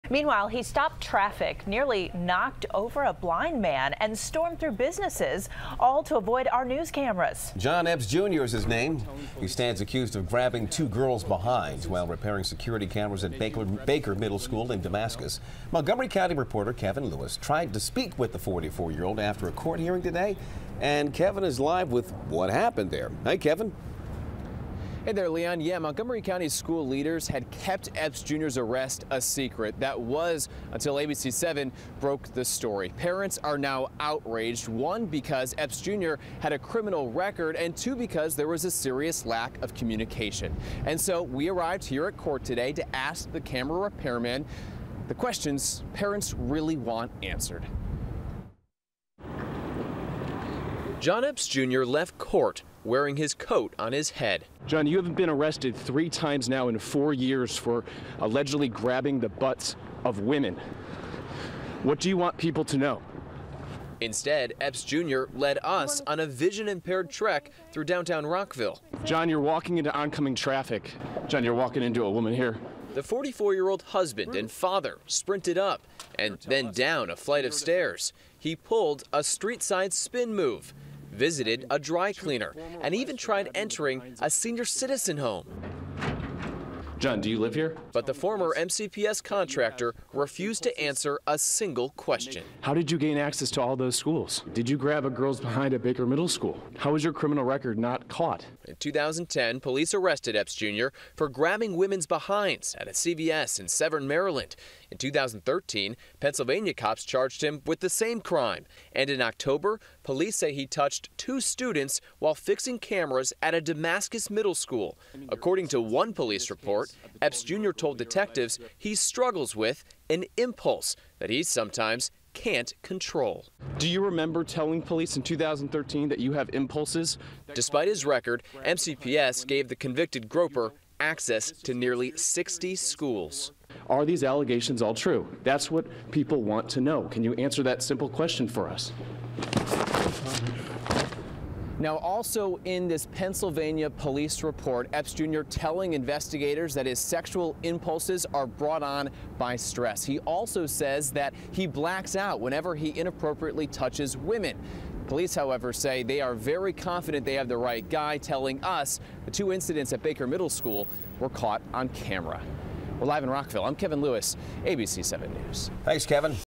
Accused butt grabber dodges news cameras
John Epps Jr., 44, of Fort Washington, is accused of grabbing two female students' behinds while maintaining security cameras at Baker Middle School in Damascus.